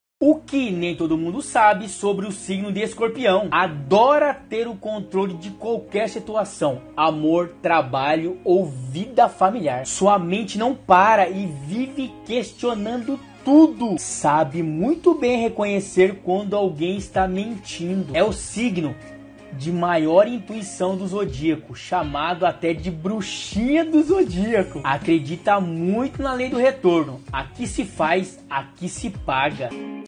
Signo ESCORPIÃO Personalidade
É introspectivo, pensador, sensível, orgulhoso, determinado, obstinado e lógico. Com tudo isso, não poderia ser uma pessoa relaxada. Tormento seria a palavra mais adequada para definir seus sentimentos. Escorpião é também o mais independente dos signos, mas precisam e lutam pelo poder custe o que custar.